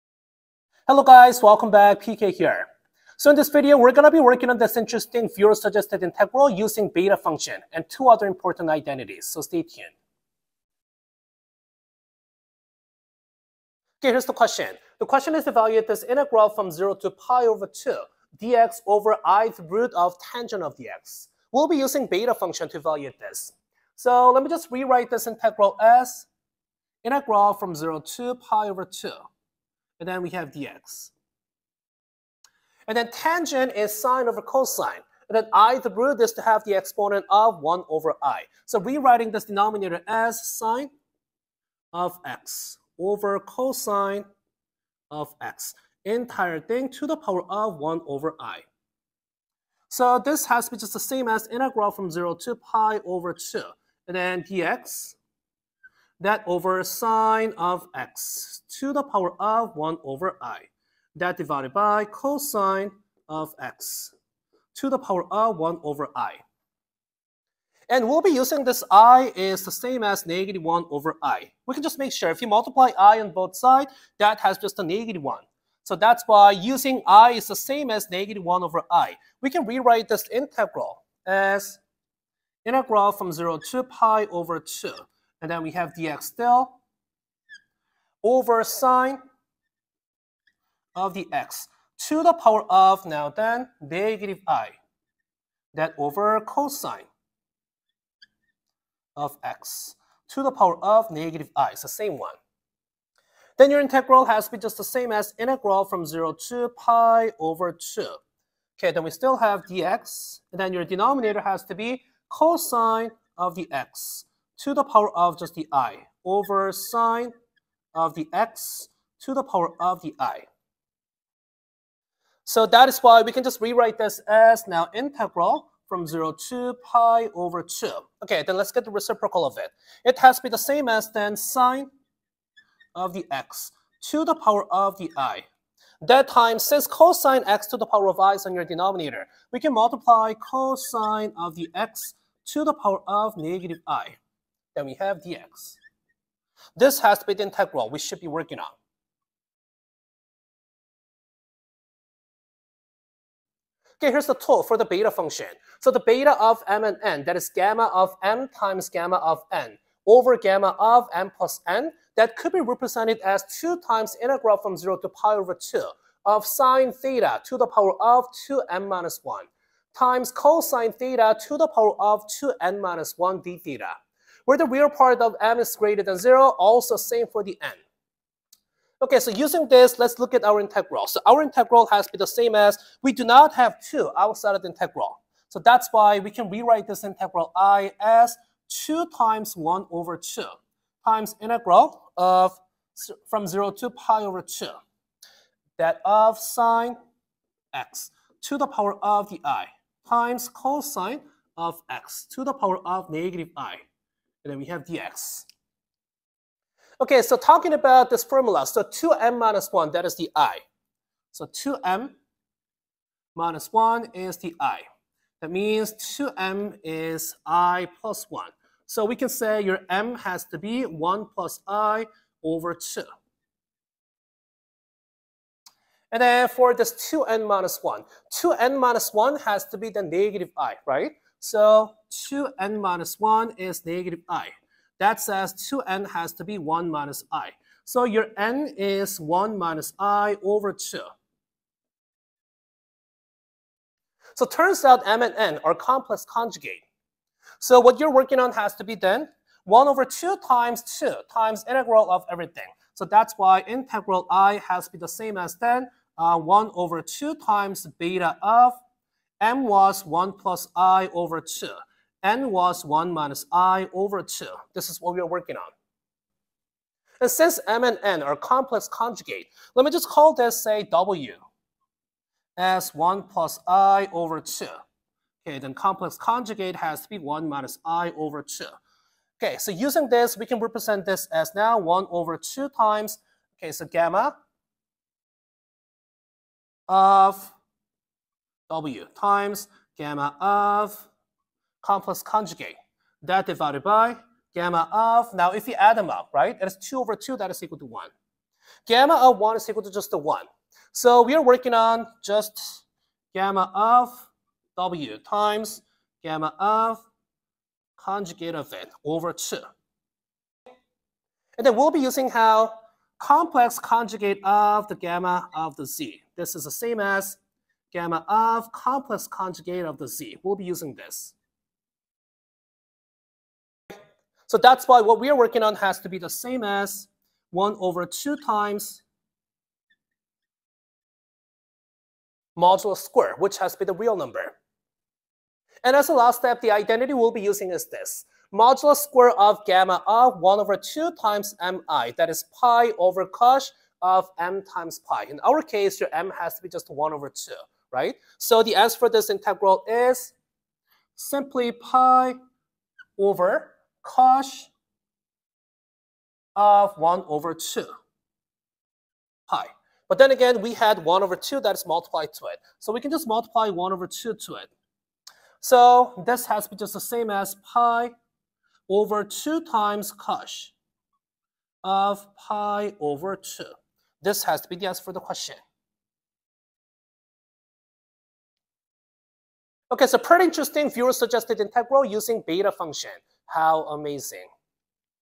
Beta Function is the Ultimate Integration Weapon
In this video, I am evaluating this interesting but challenging integral using Beta function.